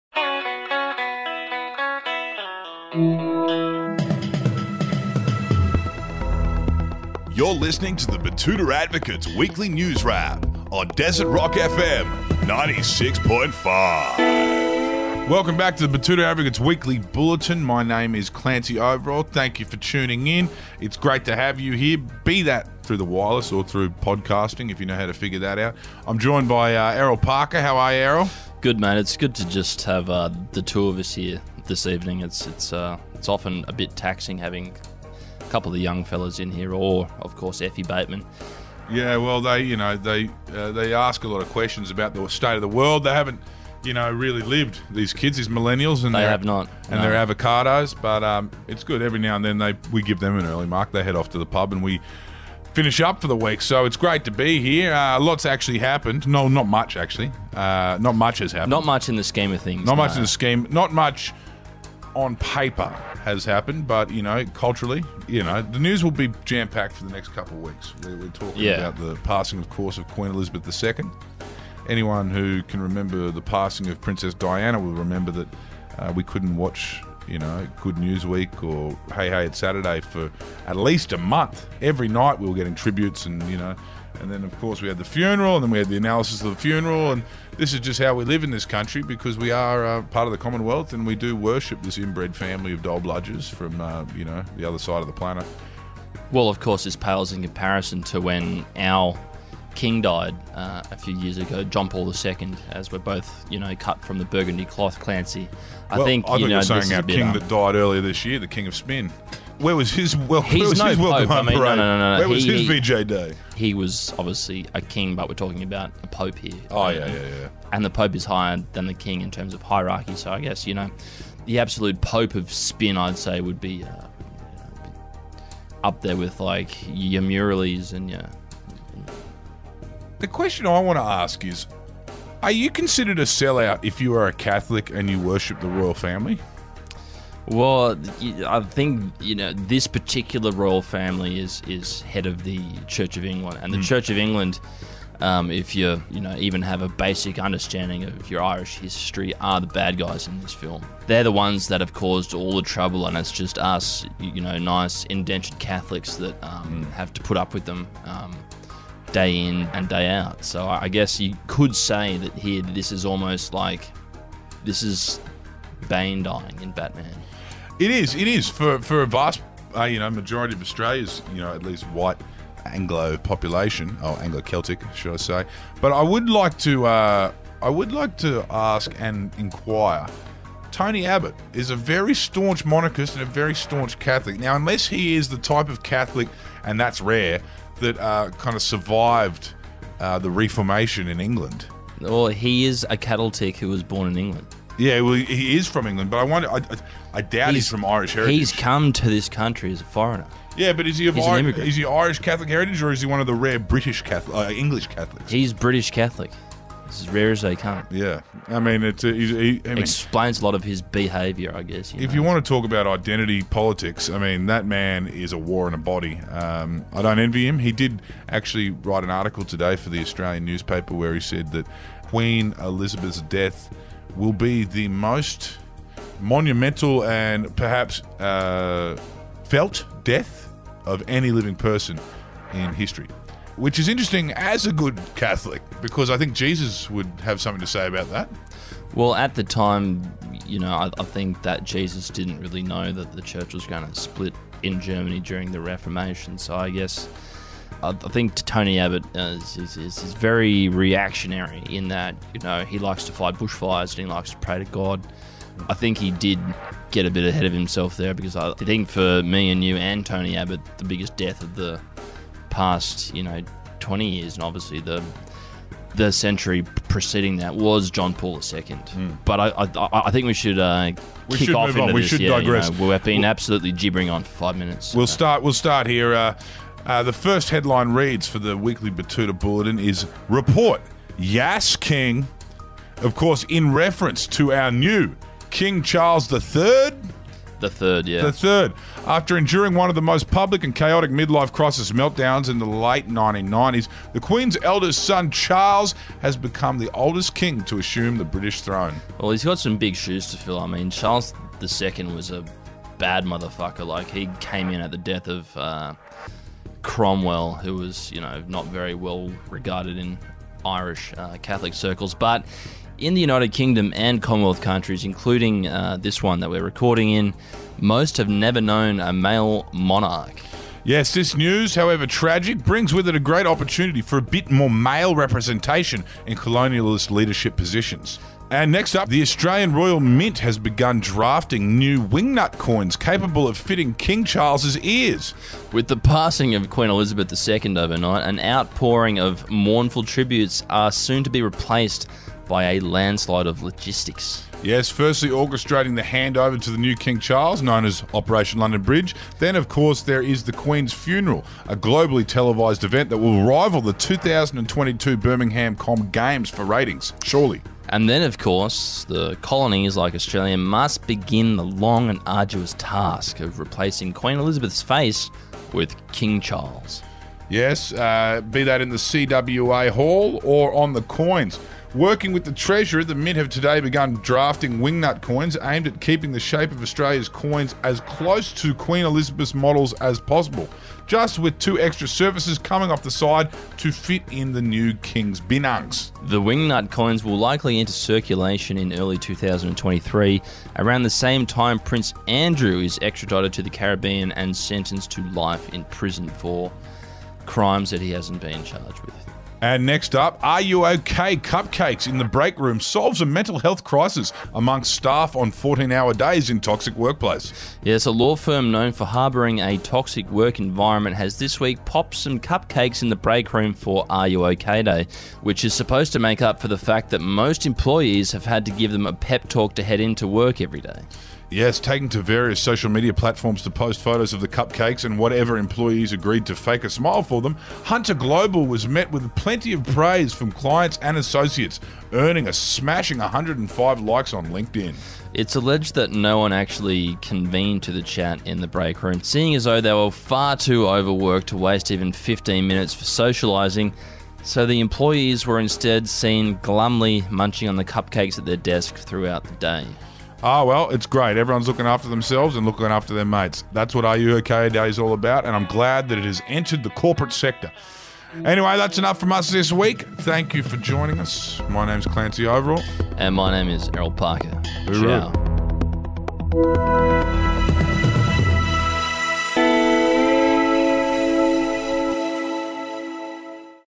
Yass King, An Update & An Easy Fix - September 9
Clancy Overell and Errol Parker wrap up all the biggest stories from the week - live from the Desert Rock FM studio in downtown Betoota.